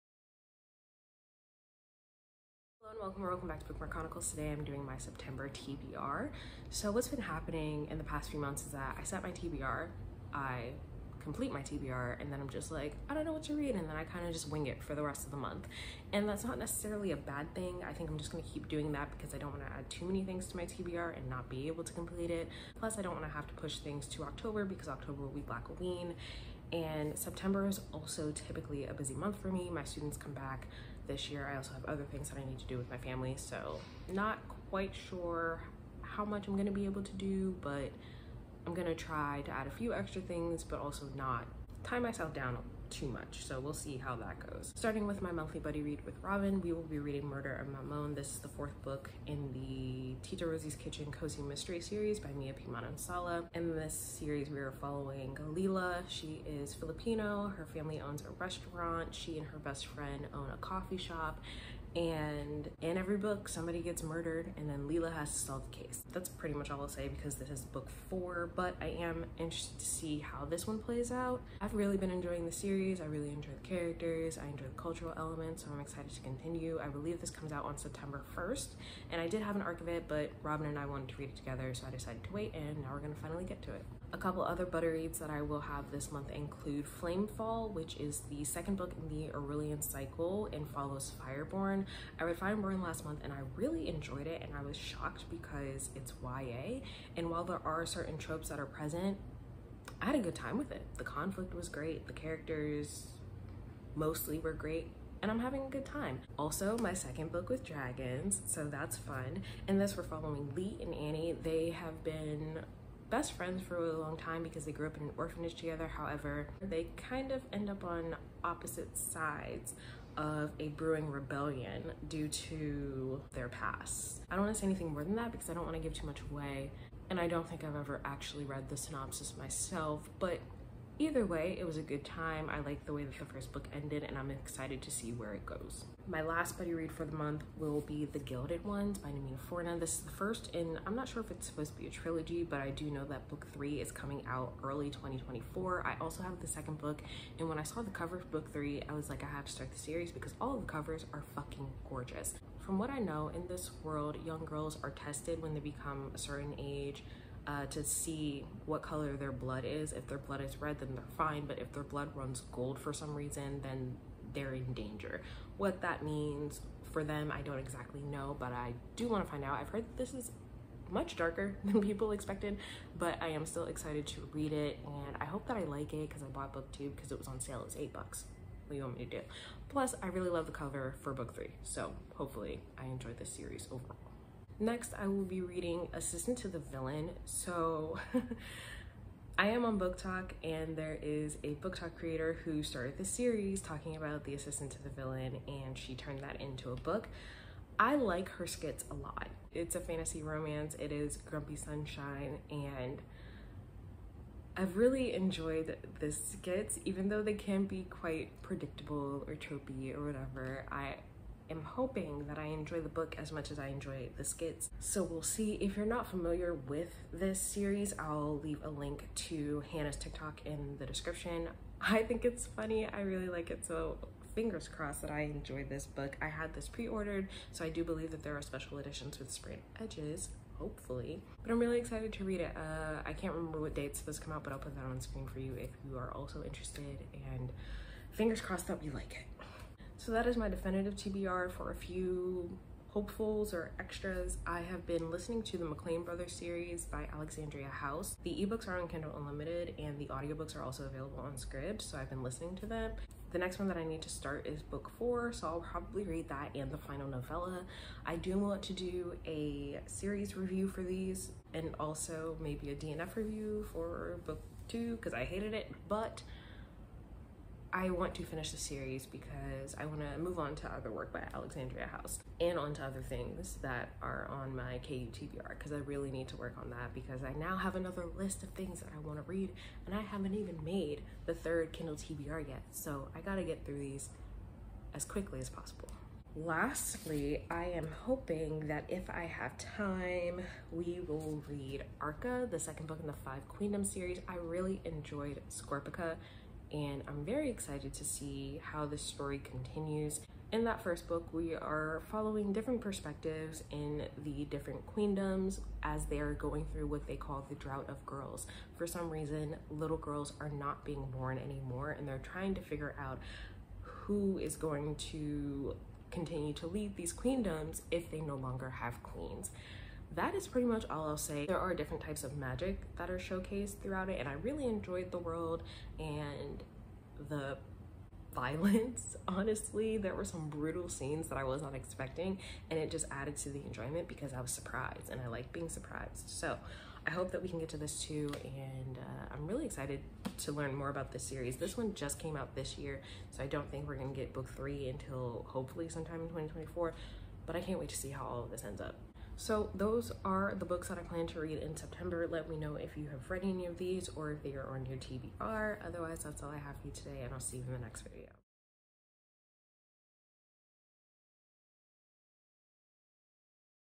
September TBR | 2023 [CC]
What are you reading this month?

Books mentioned:
Murder and Mamon
Flamefall
The Gilded Ones
Assistant to the Villain
The McClain Brothers
Arca